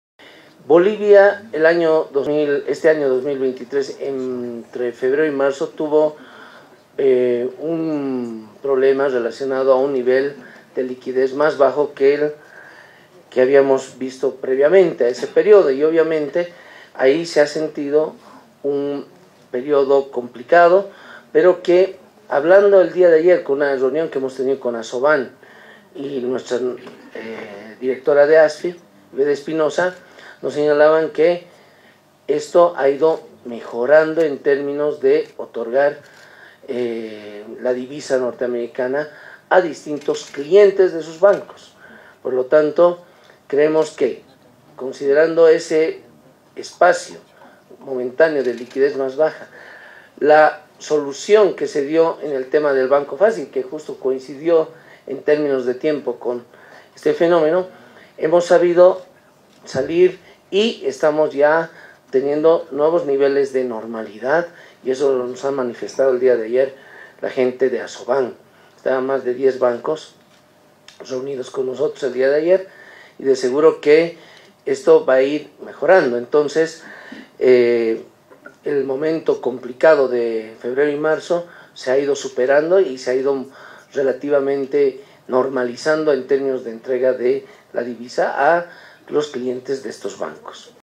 Tras reunión con ASOBAN y ASFI se asegura que paulatinamente se va normalizando la liquidez del
Ministerio de Economía y Finanzas Públicas: Tras reunión con ASOBAN y ASFI se asegura que paulatinamente se va normalizando la liquidez del dólar en el sistema financiero.

El  Ministro de Economía y Finanzas Públicas Marcelo Montenegro. Dijo que tras tener una reunión con ASOBAN y ASFI  a seguro que se va normalizando  la liquidez de dólar en el sistema financiero nacional que se está otorgando  la divisa norteamericana a distintos clientes de los diferentes bancos que van registrando nuevos niveles de normalidad en liquidez.